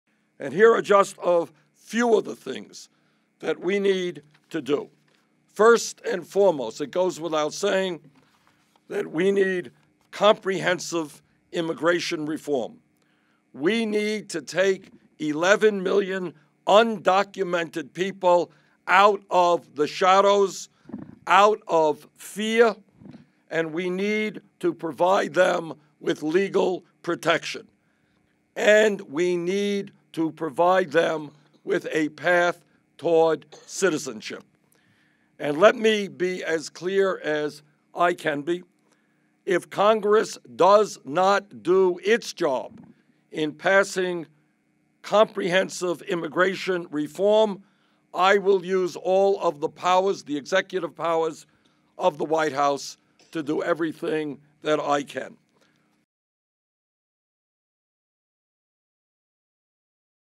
Sanders Visits U.S.-Mexican Border | Bernie Sanders
U.S. Sen. Bernie Sanders made a visit to the U.S.-Mexican border on March 19. Sanders was joined during the visit by Rep. Raúl Grijalva, Cook County Commissioner Jesús “Chuy” Garcia, Santa Cruz County Sheriff Tony Estrada and DREAMers Jessica Elizabeth Orellana Díaz and Julio Zuniga.